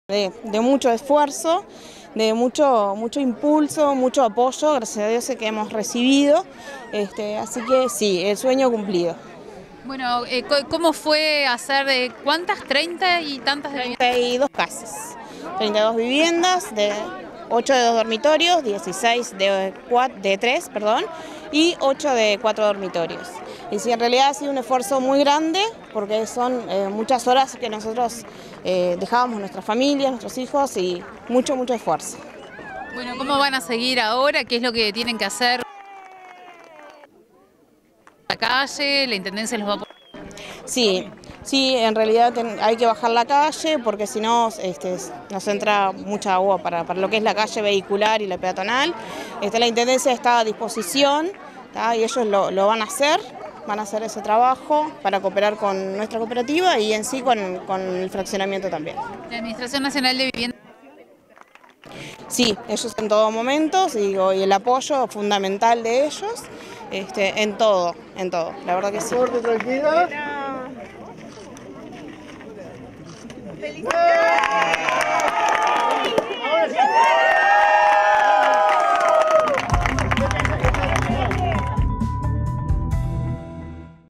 VIVIENDAS | Mónica de los Santos - Pte. Cooperativa Covisueño 2013.
Se cumplió el sueño de la casa propia para 32 familias. Viviendas realizadas bajo el régimen de autoconstrucción